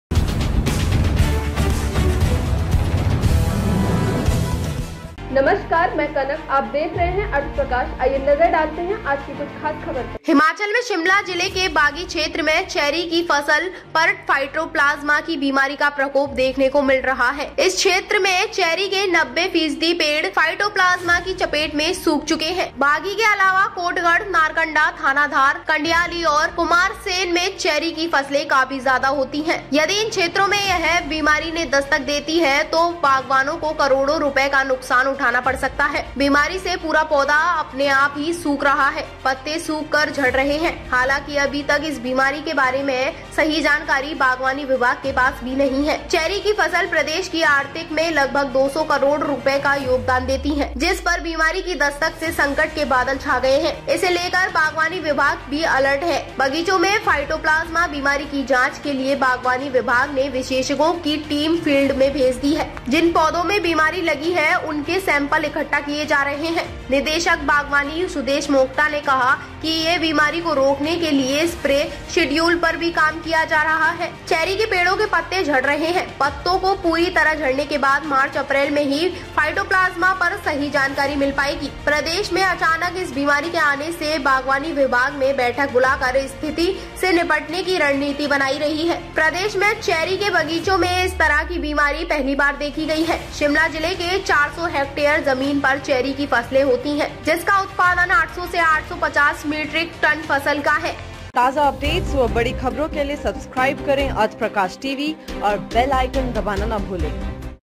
शिमला जिला के बाघी क्षेत्र में चेरी की फ़सल पर फाइटोप्लाजमा बीमारी का प्रकोप देखने को मिल रहा है
हिमाचल के शिमला जिला के बाघी क्षेत्र में चेरी की फ़सल पर फाइटोप्लाजमा बीमारी का प्रकोप देखने को मिल रहा है। इस क्षेत्र में चेरी के 90 फीसदी पेड़ फाइटोप्लाजमा की चपेट से सूख चुके हैं।

बाघी के अलावा कोटगढ़, नारकंडा, थानाधार, कंडियाली, और कुमारसैन में चेरी की फसल काफी ज़्यादा होती है। यदि इन क्षेत्रों में यह बीमारी ने दस्तक देती है तो बागवानों को करोड़ों रुपए के नुकसान उठाना पड़ सकता है।बीमारी से पूरा पौधा अपने आप ही सूख रहा है। पत्ते सूख कर झड रहें हैं। हालांकि अभी तक इस बीमारी के बारे में सही जानकारी बागवानी विभाग के पास भी नही है। चेरी की फसल प्रदेश की आर्थिकी में लगभग 200 करोड़ का योगदान देती है। जिस पर बीमारी की दस्तक से संकट के बादल छा गए हैं।

इसे लेकर बागवानी विभाग भी अलर्ट पर है। बगीचों में फाइटोप्लाजमा बीमारी की जांच के लिए बागवानी विभाग ने विशेषज्ञों की टीम फील्ड में भेज दी है। जिन पौधो में बीमारी लगी है उनके सैंपल इकट्ठा किए जा रहे हैं।निदेशक बागवानी सुदेश मोक्टा ने कहा कि बीमारी को रोकने के लिए स्प्रे शेड्यूल पर भी काम किया जा रहा है। चेरी के पेड़ों के पत्ते झड़ रहे हैं। पत्तों के पूरी तरह झड़ने के बाद मार्च-अप्रैल में ही फाइटोप्लाजमा पर सही जानकारी मिल पाएगी। प्रदेश में अचानक इस बीमारी के आने से बागवानी विभाग ने बैठक बुलाकर स्थिति से निपटने की रणनीति बनाई है।

प्रदेश में चेरी के बगीचों में इस तरह की बीमारी पहली बार देखी गई है। शिमला जिला के 400 हेक्टेयर ज़मीन पर चेरी की फ़सल होती है। जिसका उत्पादन 800 से 850 मीट्रिक टन फ़सल का है।